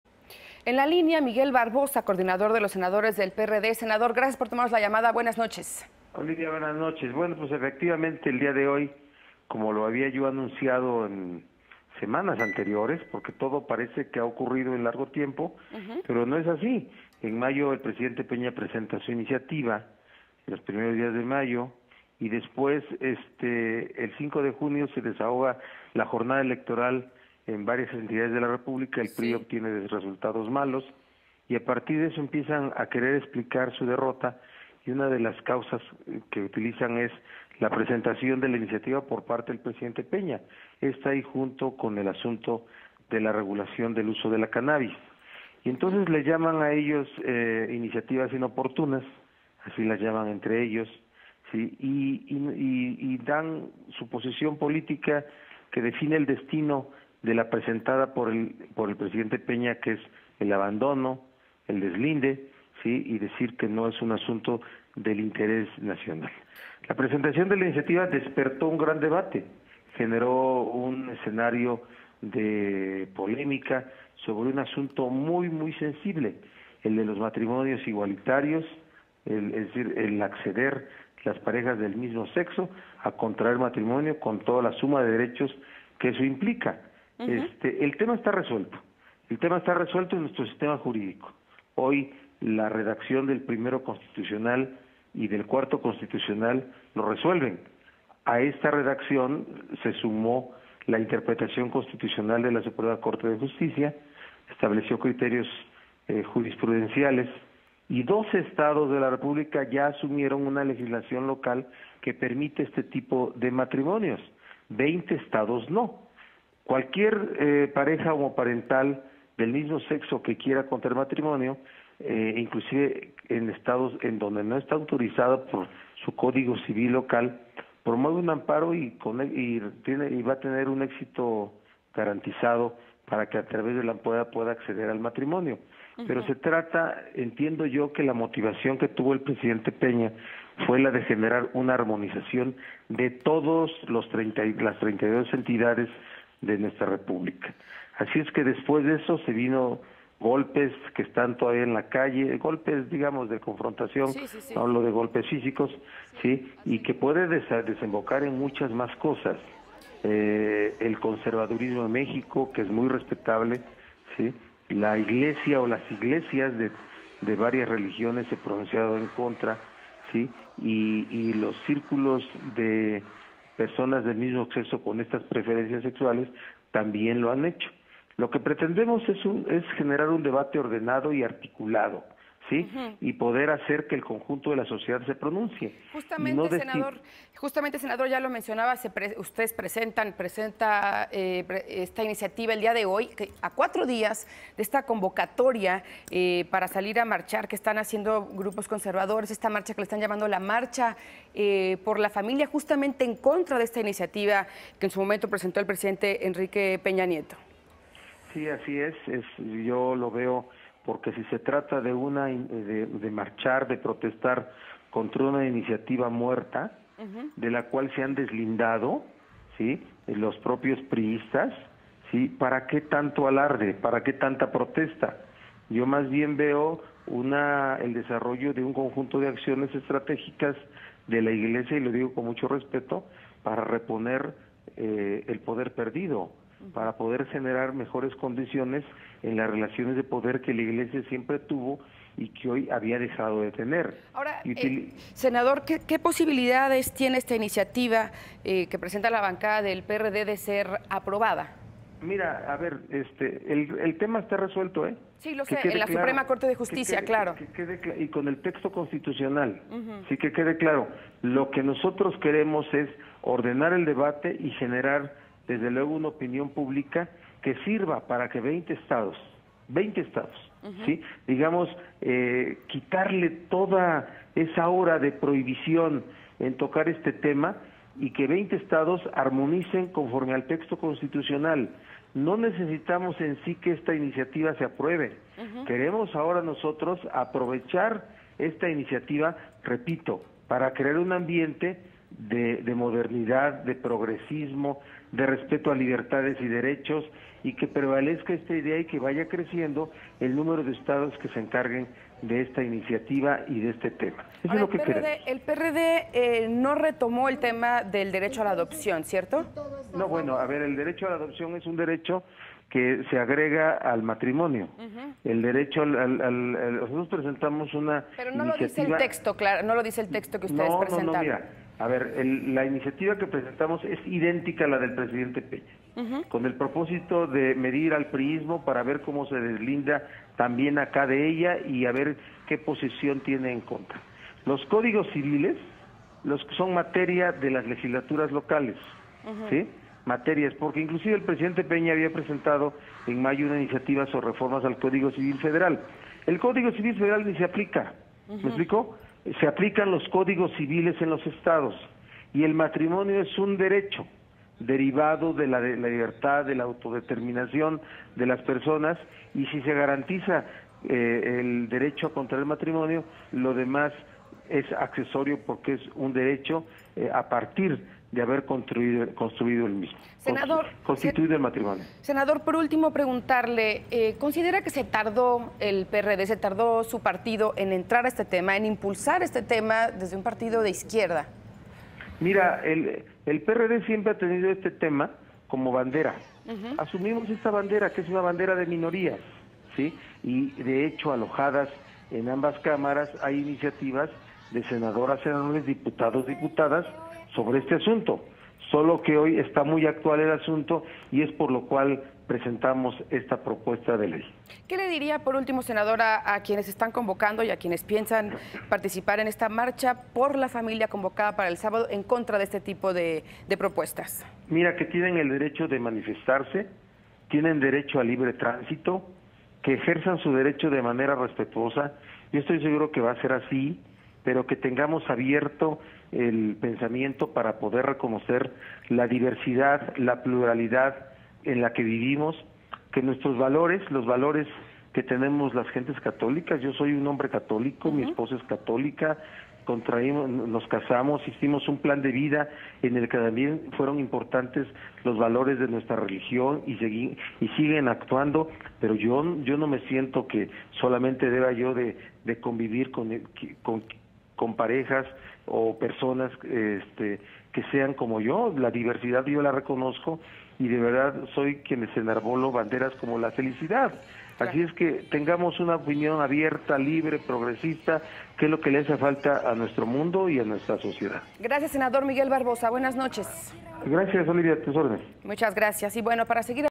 Opinión libre y progresista le hace falta al mundo y a la sociedad: Barbosa
Miguel Barbosa, coordinador de la bancada de Senadores del PRD conversó con Olivia Zerón acerca de la propuesta de matrimonios igualitarios.